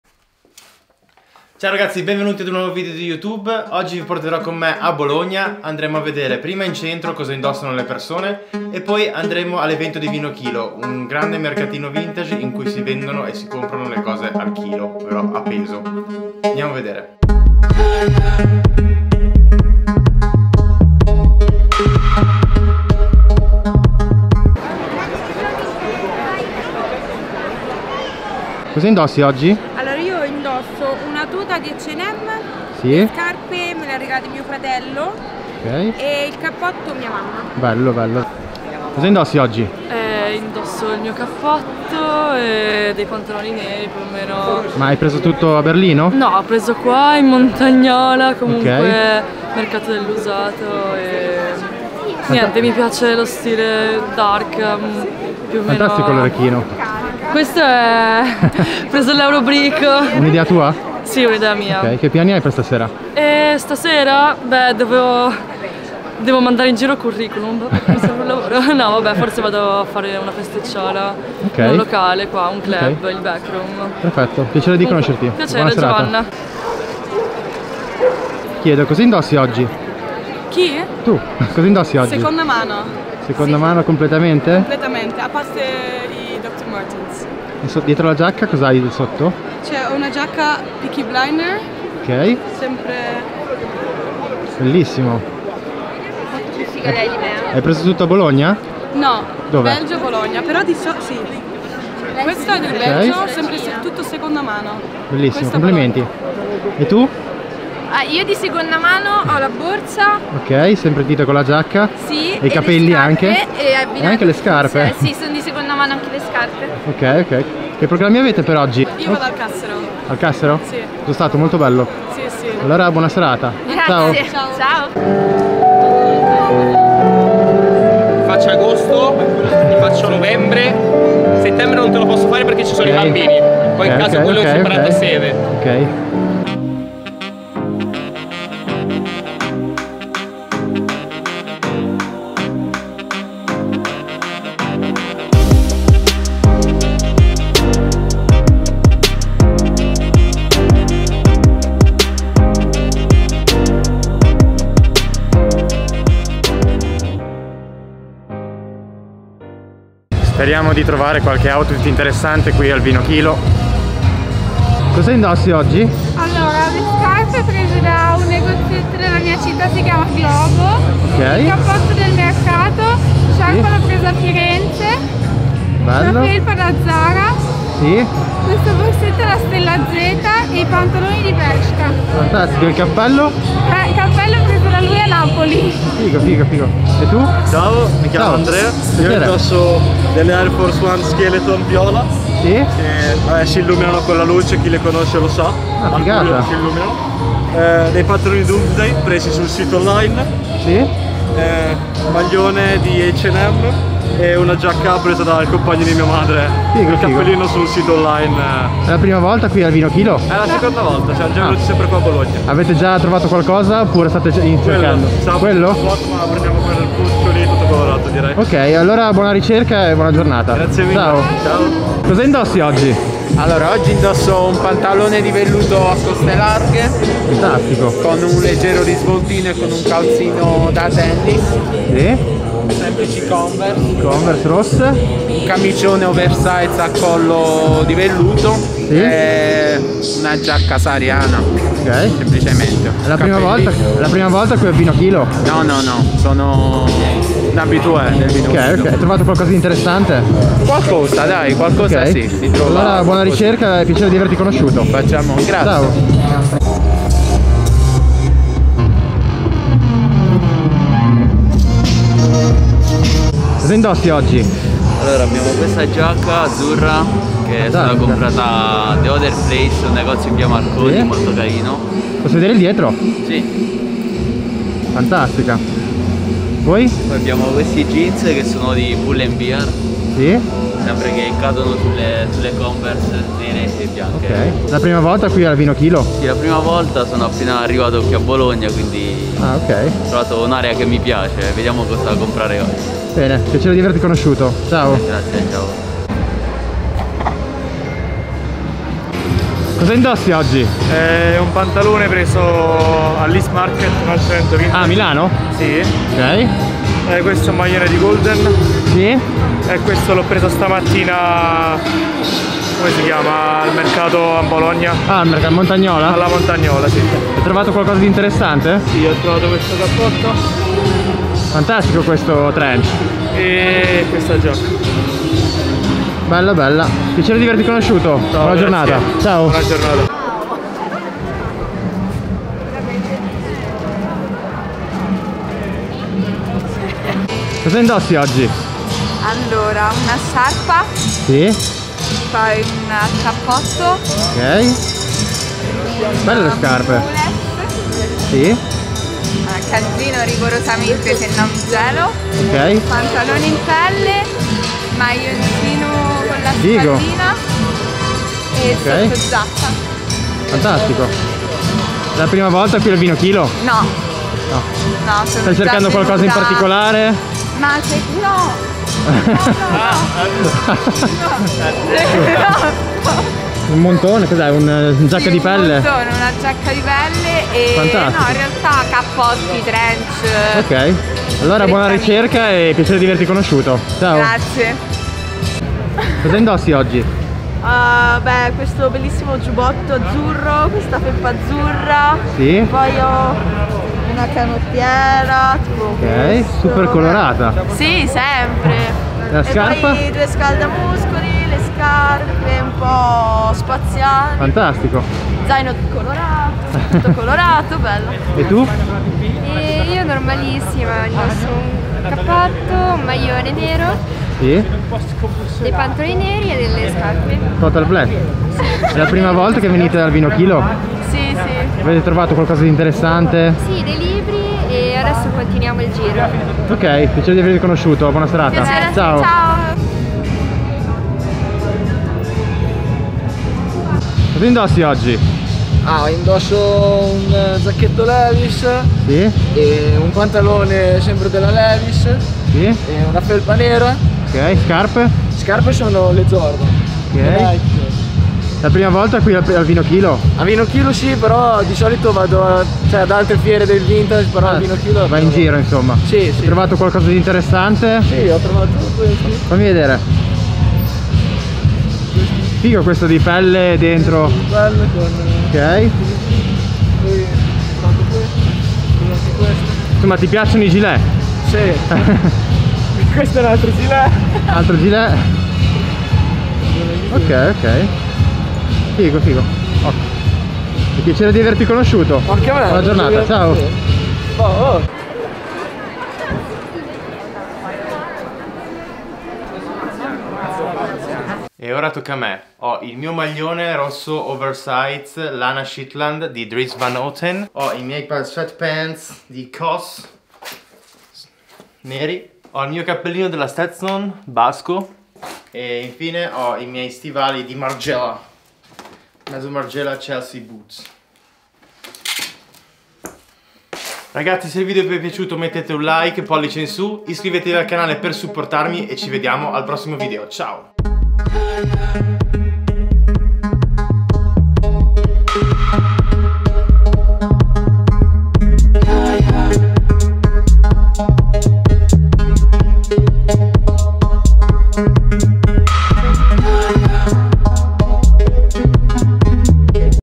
Cosa indossano le persone a BOLOGNA?ft VINOKILO | EP 3[4K]
Cosa indossano le persona in ITALIA?Sono stato a Bologna per scoprirlo all evento VINOKILO

What Are People Wearing in ITALY? @ Bologna,VINOKILO

VINOKILO:
Per gli amanti del vintage, l'inverno è la stagione ideale e Vinokilo è il paradiso. Si tratta di un pop-up store, un negozio temporaneo, e non uno qualsiasi, ma il più grande d'Europa. Sarà ospitato, come la precedente edizione, negli spazi del DumBo, in via Casarini 19.
Vinokilo è un esperimento di economia circolare: si può comprare, ma si può anche partecipare attivamente portando vestiti dismessi. Si ottiene così uno sconto sui nuovi acquisti fino al 30%. Ma la sua unicità sta nel modo in cui si compra: i pezzi vengono pagati "al kilo". Sarà possibile trovare tantissimi pezzi unici di abbigliamento vintage, migliaia di alternative di capi divisi per tipologie, come cappotti, trench, bomber, camicie fantasy e di flanella, jeans, pantaloni, giacche e tanto altro ancora. Una grande varietà di brand, anni, stili e taglie, dalla XXS alla plus. Ci saranno anche anche accessori unici e rari, come borse in pelle, scarpe, cinture e molto altro. Un ventaglio davvero vasto di possibilità in cui perdersi, per rinnovare l'armadio
Vinokilo unisce l'utile al dilettevole. Sostenere il mercato dell'usato significa aiutare l'ambiente, limitare gli sprechi indotti dall'industria del fast fashion e promuovere uno stile di vita più sostenibile. Oltre allo shopping ci saranno anche altre attività di intrattenimento, ludiche e creative. Non mancheranno musica live, dj set e workshops per confrontarsi con artisti e imprenditori. Verranno affrontati temi di attualità come l'educazione civica, la trasformazione green, la digitalizzazione, la sostenibilità e le pratiche di economia circolare. Sarà presente anche un'area 'food and drinks'